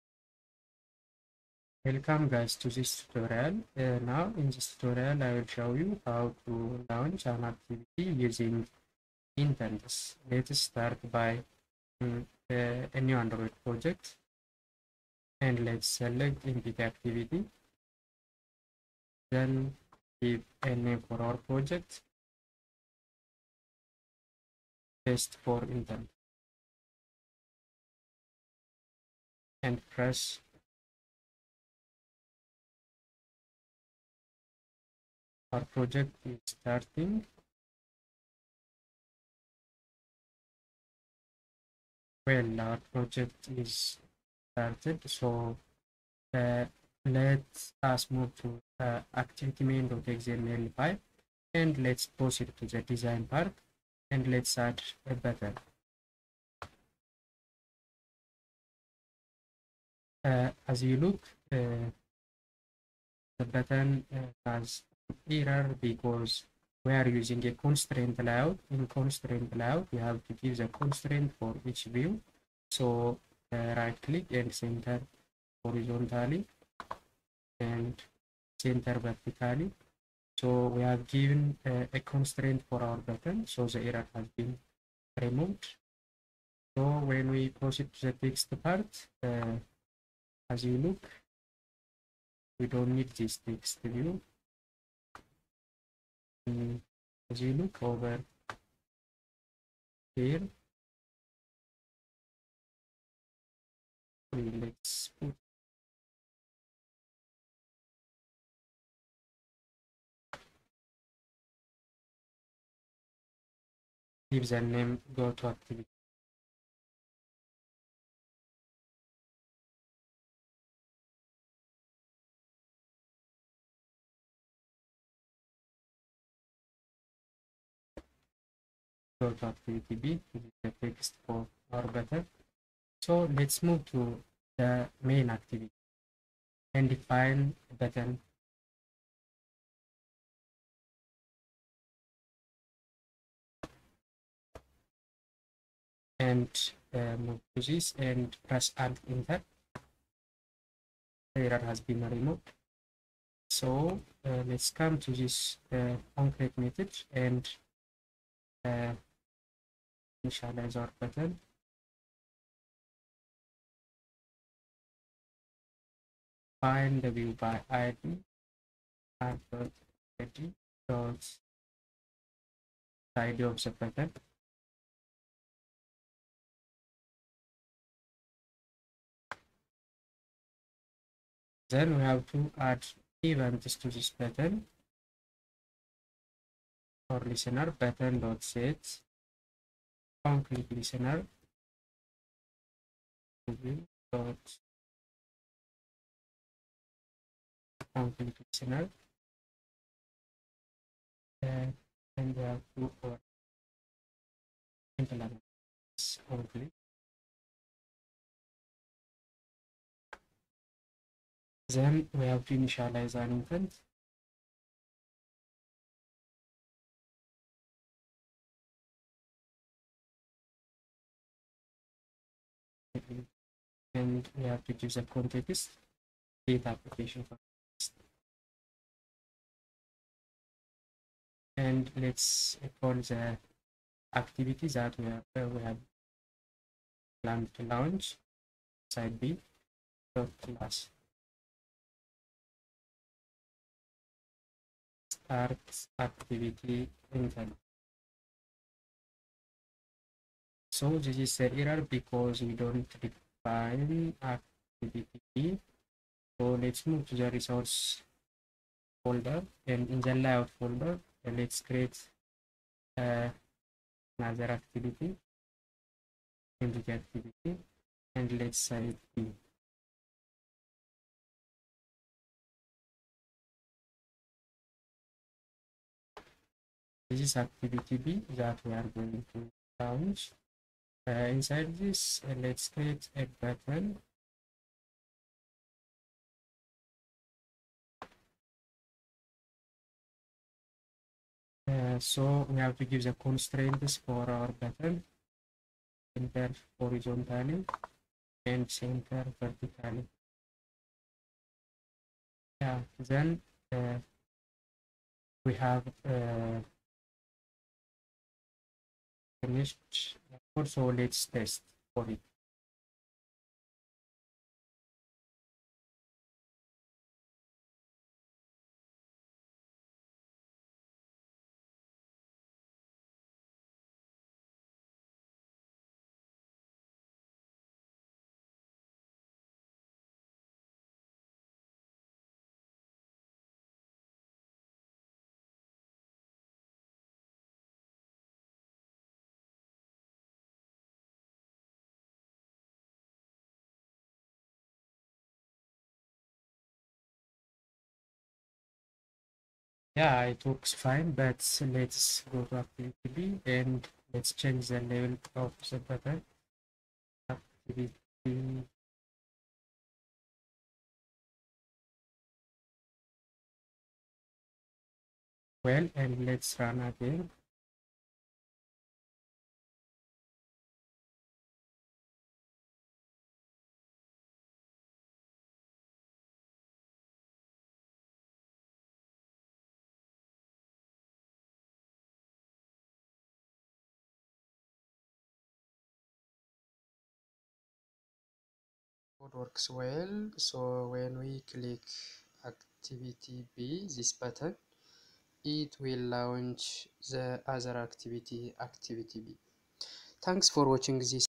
How to use Intent to open an Activity, Android programming
Showing How to use Intents to open Activity for a Button click in Android Programming .